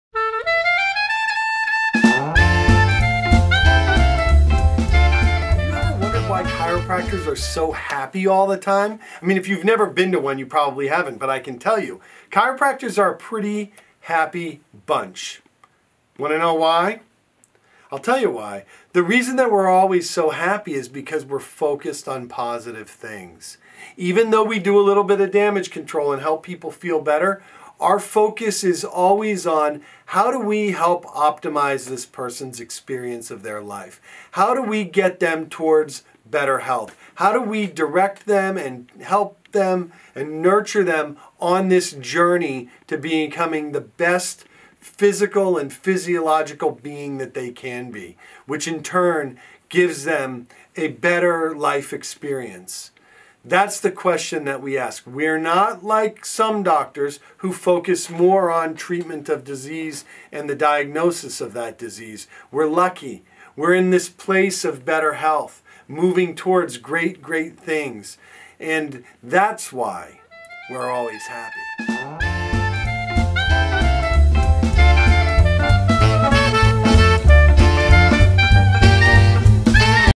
Why Are Chiropractors Always So Happy? Dr. Lou in Portland, Maine Explains!
Ever notice that chiropractors are all happy? What is the secret they hold to great moods and attitudes? What is it about life that they understand? Can their mood and attitude rub off on you? How do you become a happier person? Dr. Lou can help you find greater happiness!

Dr. Lou Jacobs
Jacobs Chiropractic Acupuncture, P.A.
Perinatal & Pediatric Chiropractic of Maine, P.A.
138 St. John Street
Portland, Maine
04102